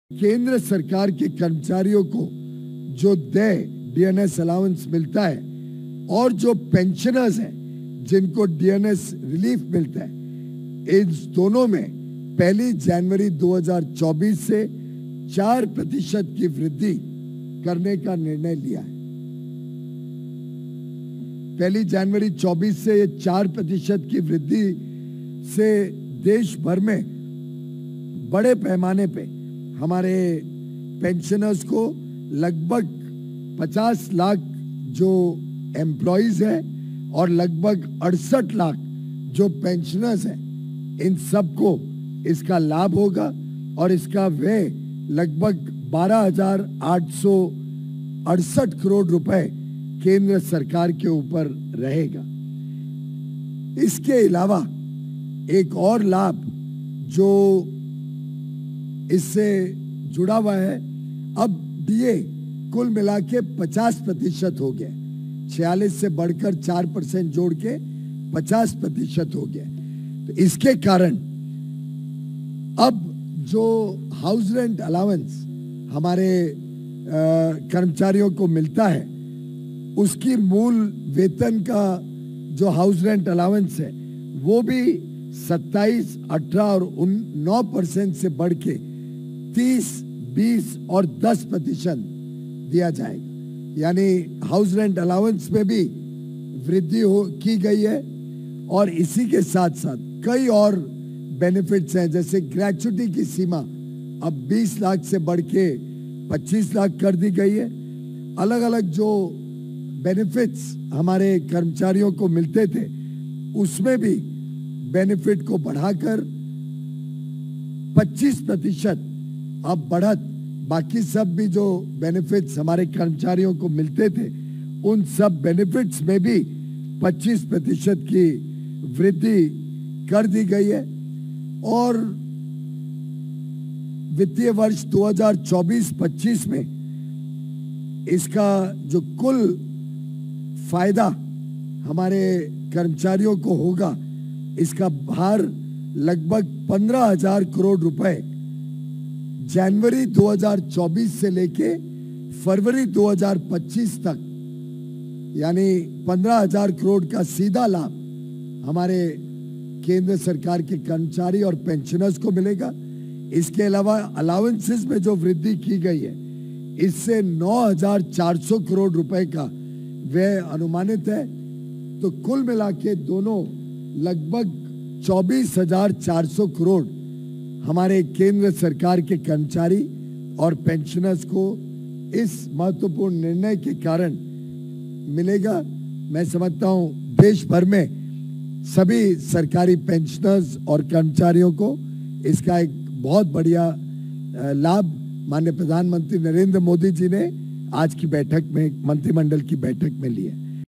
Union Minister Shri Piyush Goyal addressed media
What increased along with DA?  Union Minister Shri Piyush Goyal addressed Media